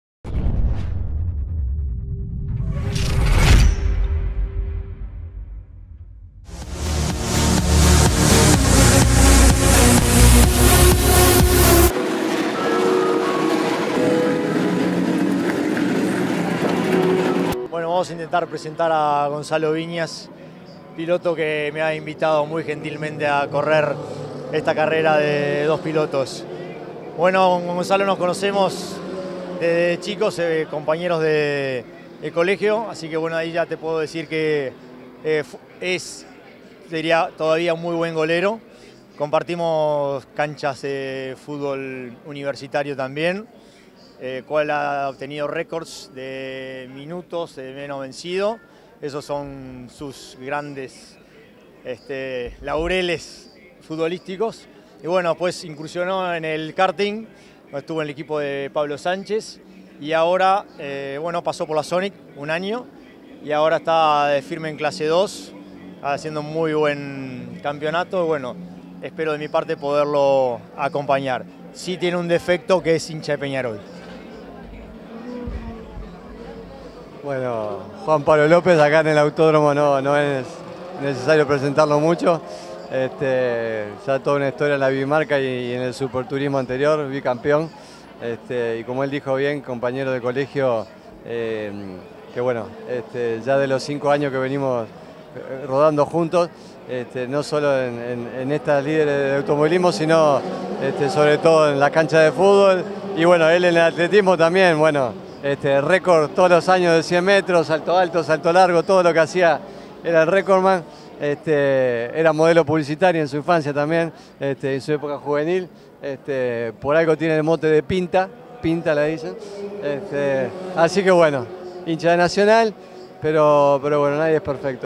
STC2 | Gonzalo Viñas - Juan P. Lopez 2018
Gonzalo Viñas y Juan Pablo Lopez llevan una amistad de años que comenzó en la niñez y ahora Juan Pablo, bicampeón de Superturismo, corre como invitado de Gonzalo en las carreras de duplas del STClase2.
Te invitamos a ver su presentación en el siguiente vídeo.